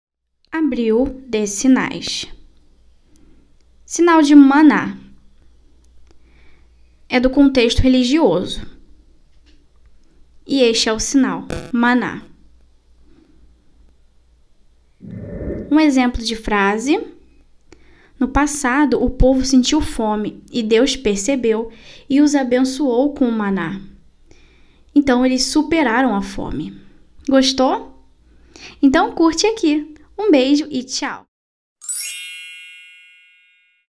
Librando! - Sinal de Maná em Libras
O maná que desceu dos céus!
Vamos aprender em Libras? “Simboraaa”
Não esqueça de curtir e compartilhar para me ajudar❤️🤟🏻🤩

*contatos no final do vídeo*